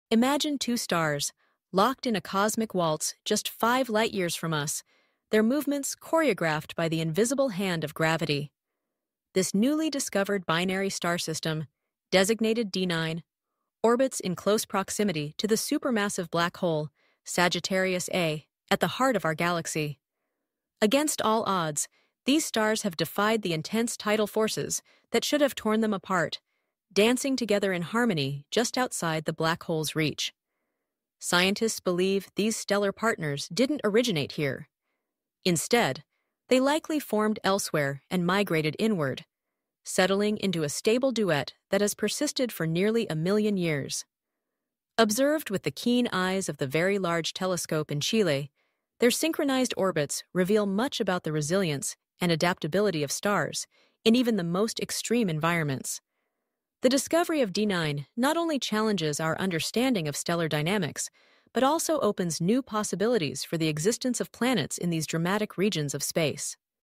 How Did These Stars Survive So Close to a Black Hole?
Witness a real-life cosmic ballet as a binary star system, orbits incredibly close to Sagittarius A, defying intense gravitational forces. This amazing stellar harmony showcases the universe's most extreme stellar dynamics. Watch to learn how these celestial wonders manage to stay together against all odds.